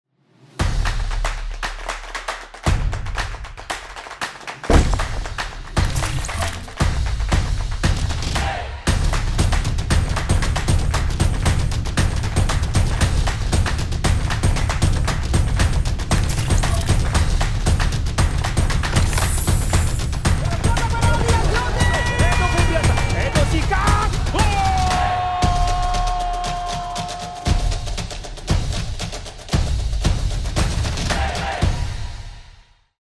Wilujeng sumping Edo Febriansyah 💙
Putra Kediri ini akan melanjutkan perantauannya bersama Pangeran Biru 🤝

Wilujeng sumping, Edo Febriansah 💙